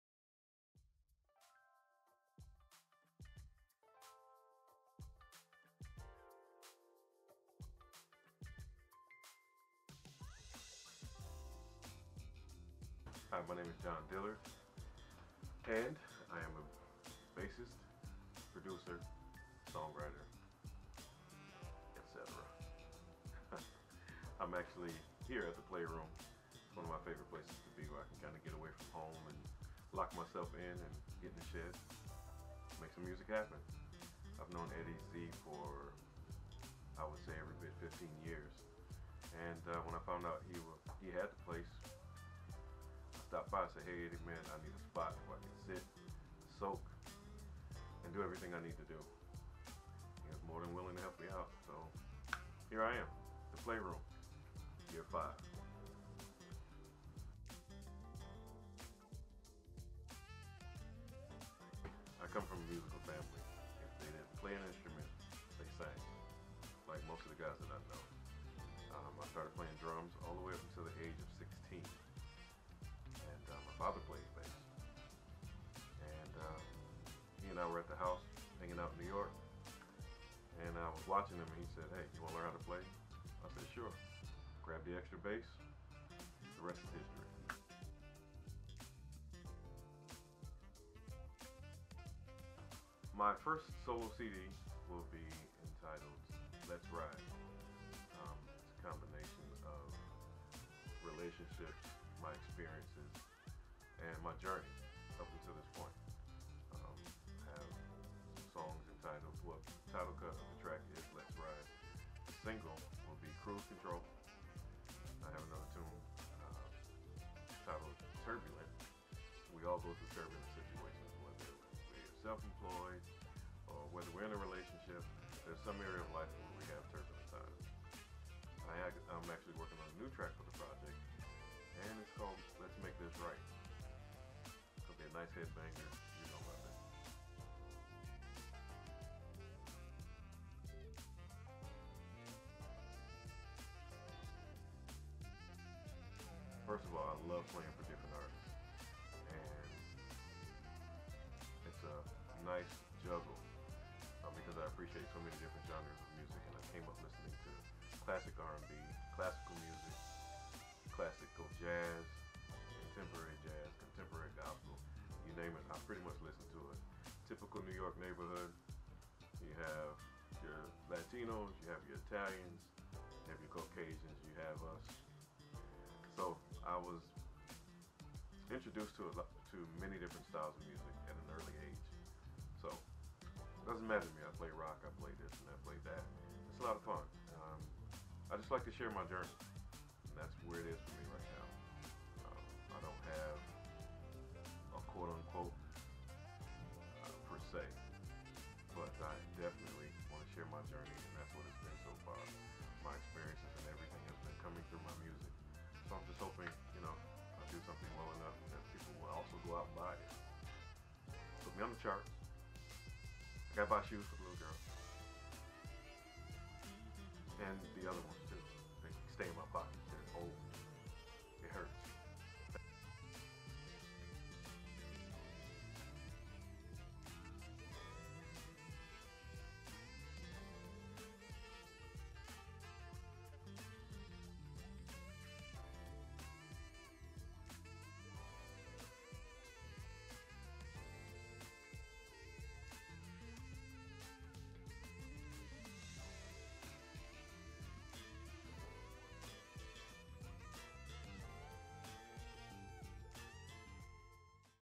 The Playroom Spotlight - John Dillard
The Playroom, located in Charlotte, NC is the safe haven for many of Charlotte's most talented musical talents. For the month of November, the Playroom would like to spotlight one its accomplished tenants. John Dillard. If you are looking for a place to rehearse